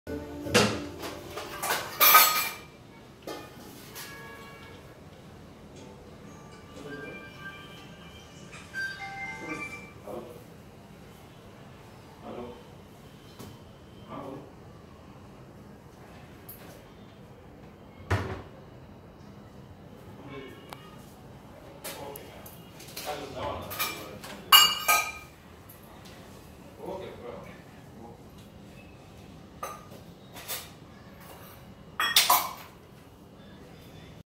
FOLDING DINING BY INSIDE INTERIOR
FOLDING DINING TABLE

 First of all it's Depand on your space  & Dimensions of your area &  carpenter is also required to do this folding table (i can provide only micanisum only)